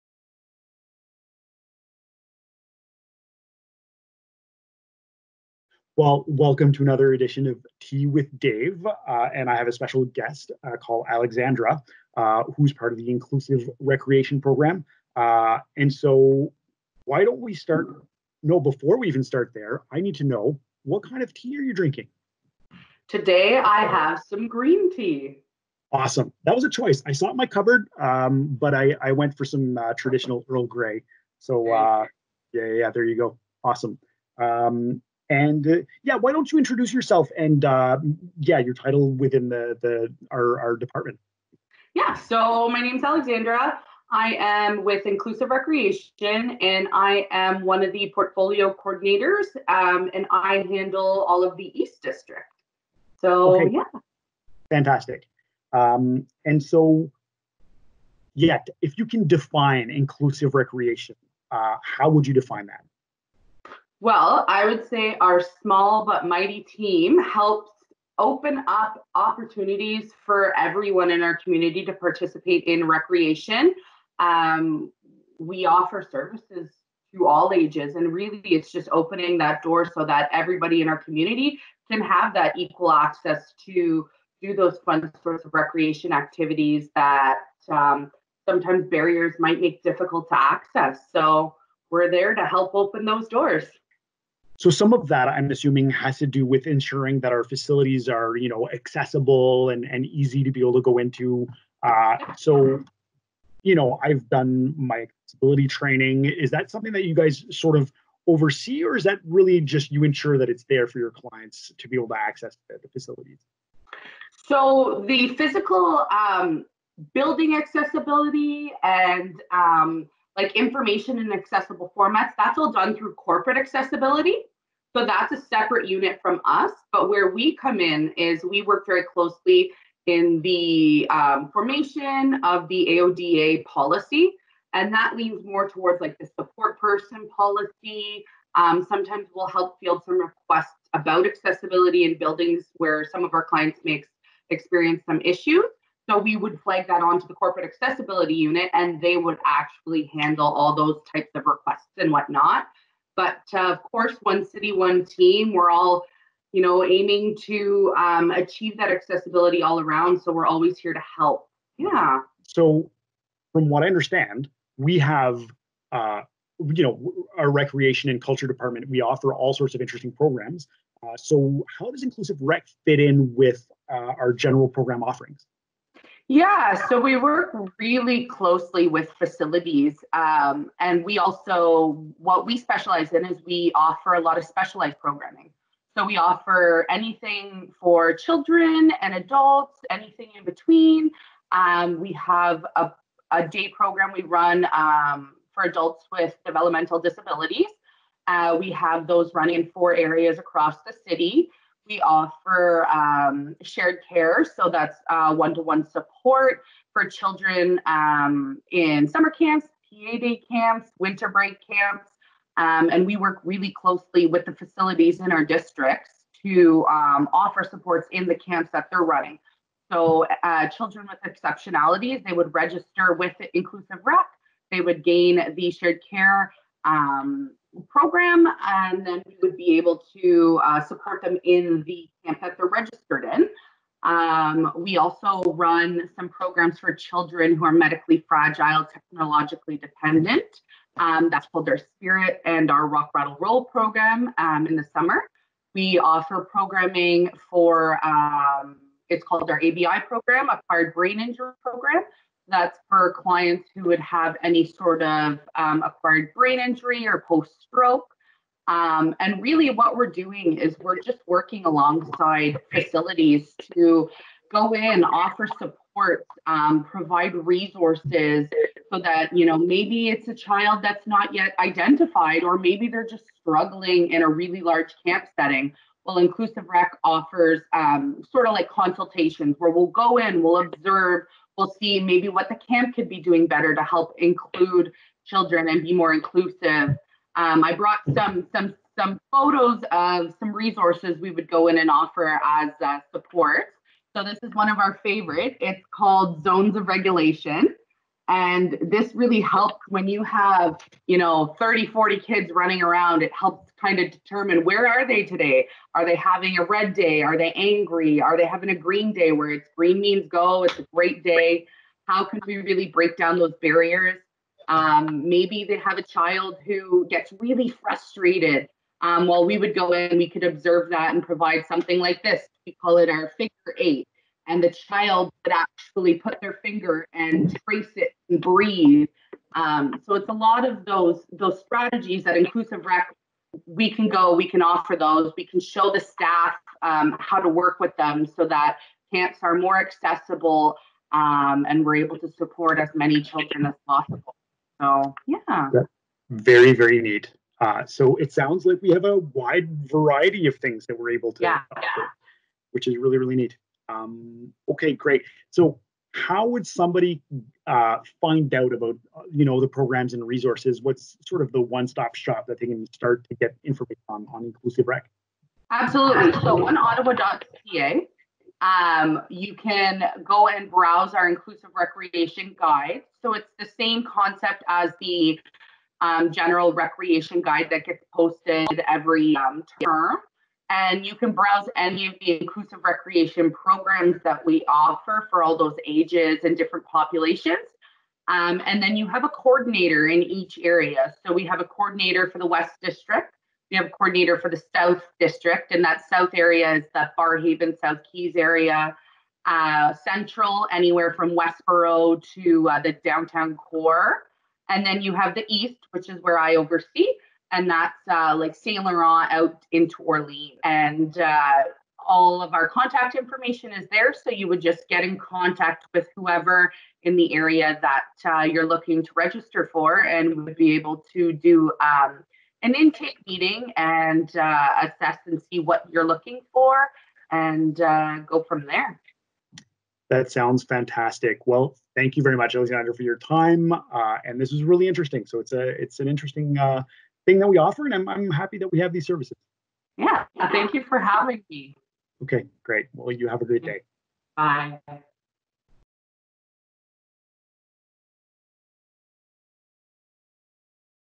Thé avec David - Loisirs inclusifs, c'est quoi? Alexandra nous en dit plus.
David discute avec Alexandra Zoryk, coordonnatrice des loisirs inclusifs à la Ville d’Ottawa, et en apprend davantage sur les programmes de loisirs spécialisés que la Ville offre aux enfants, aux jeunes et aux adultes ayant des besoins spéciaux qui les empêchent de participer pleinement aux loisirs communautaires. Ces programmes offrent aux personnes ayant une déficience intellectuelle ou physique et/ou à la santé fragile, les soutiens et les ressources dont elles ont besoin pour surmonter les obstacles.